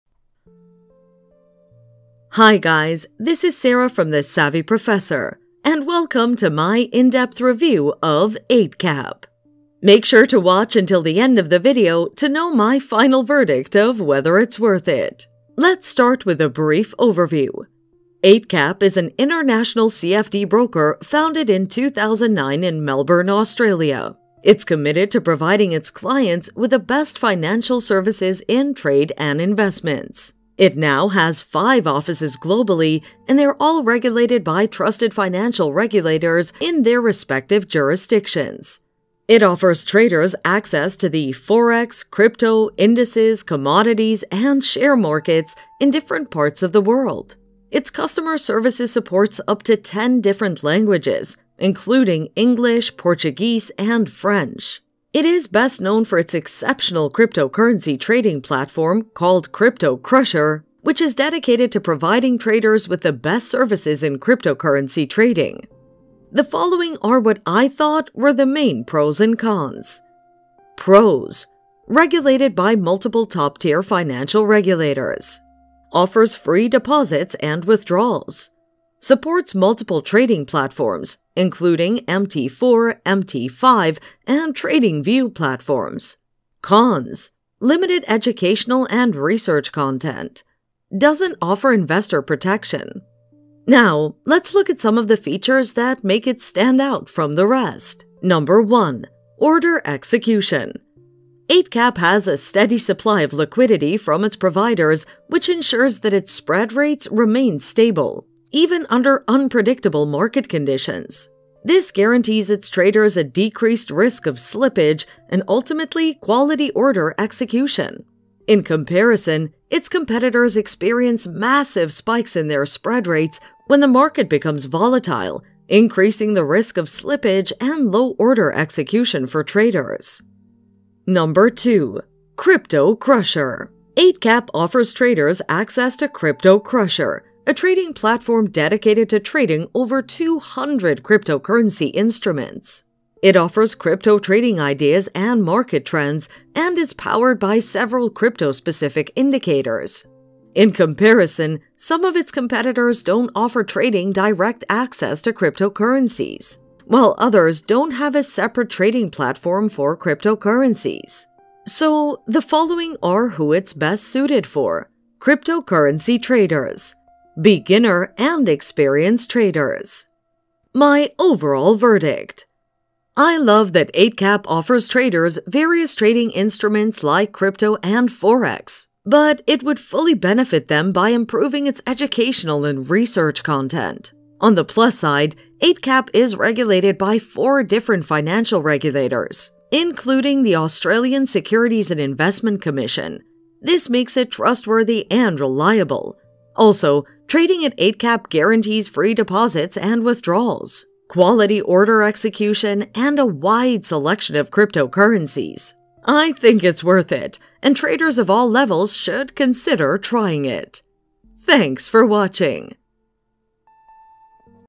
Eightcap Review - Pros and Cons of Eightcap (Can You Trust Them?)
Eightcap Review - Pros and Cons of Eightcap (Can You Trust Them?). Welcome to my in-depth review of Eightcap.

Overview of Eightcap Review:
Eightcap is an international CFD broker founded in 2009 in Melbourne, Australia. It is committed to providing its clients with the best financial services in trade and investments. It now has five offices globally, and they're all regulated by trusted financial regulators in their respective jurisdictions.